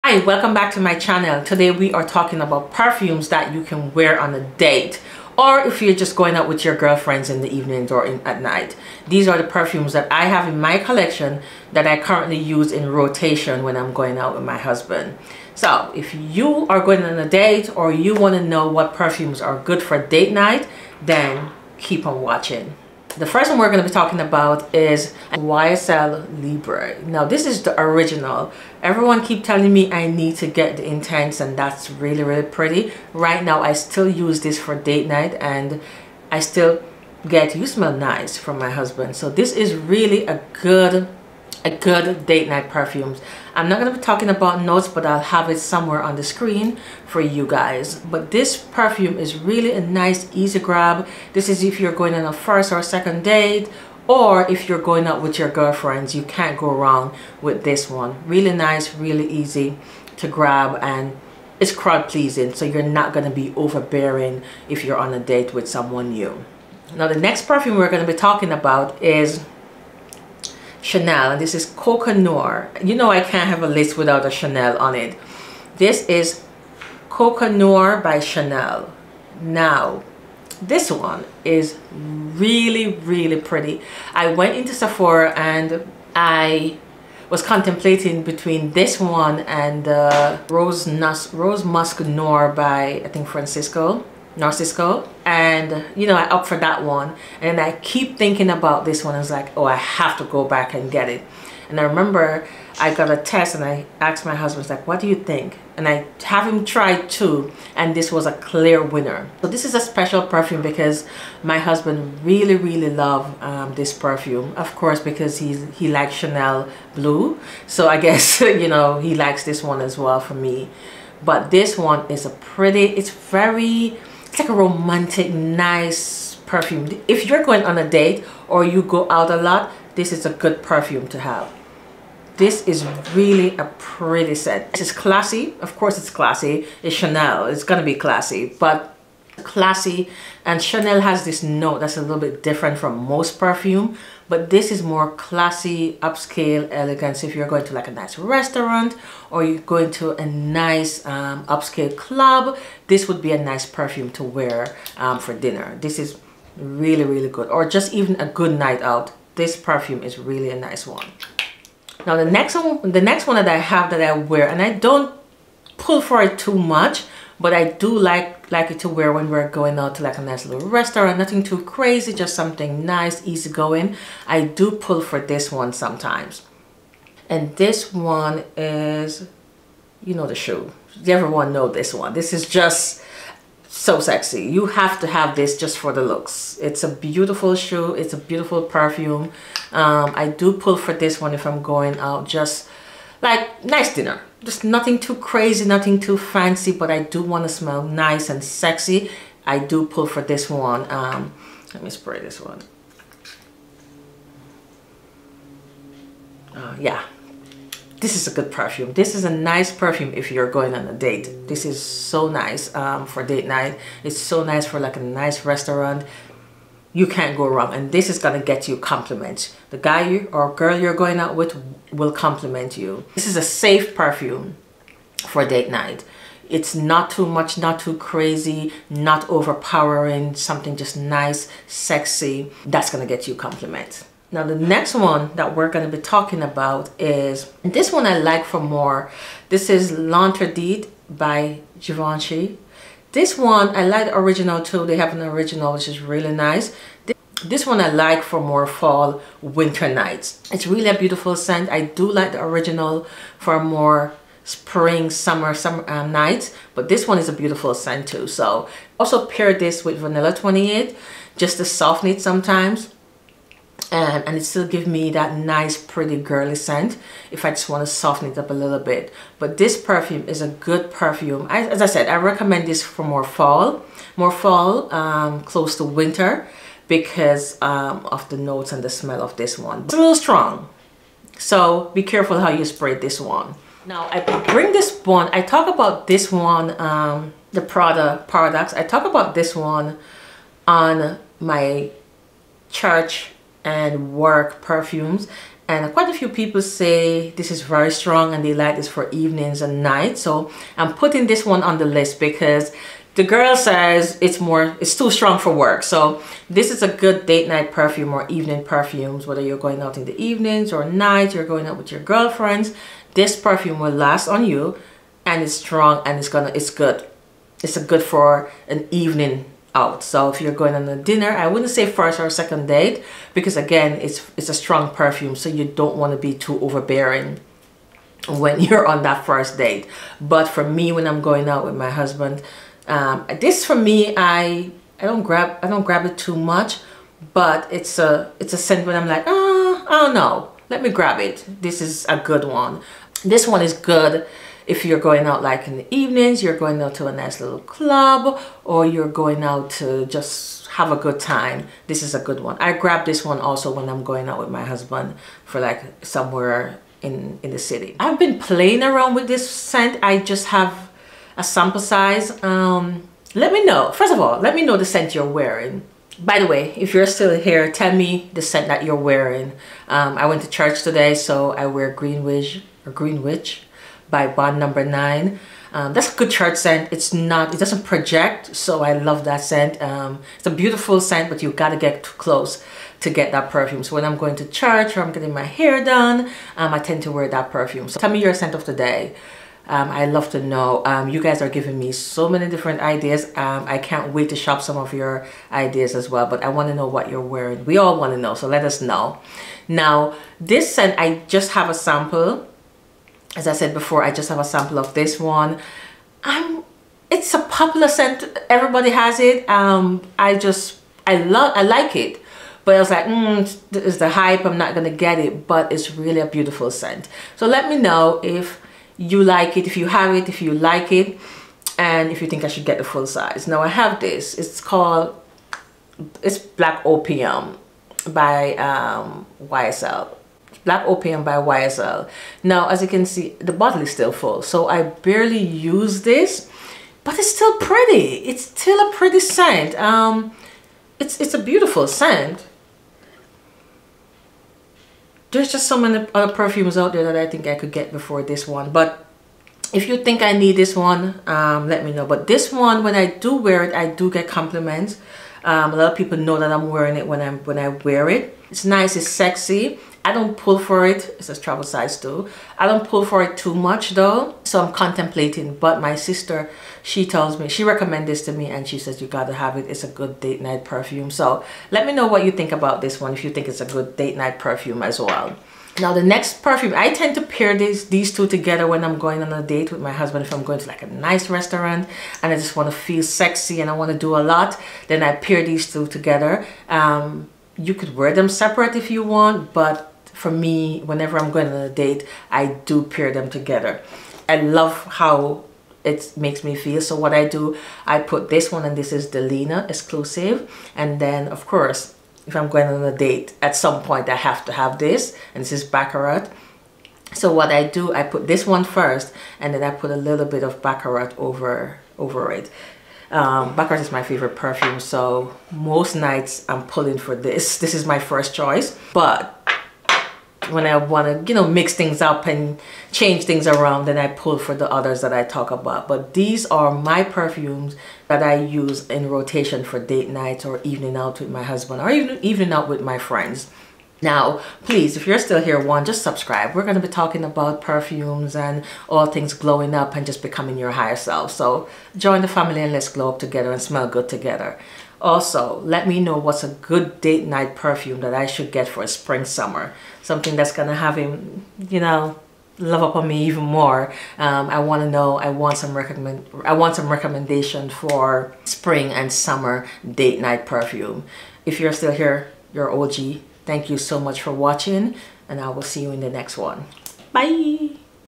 The seductive scent of romance: Date night perfumes for women
These are perfumes in my collections that can be worn on a date or just an evening out with friends.
while Baccarat is my go to, I try to incorporate other fragrance for date night.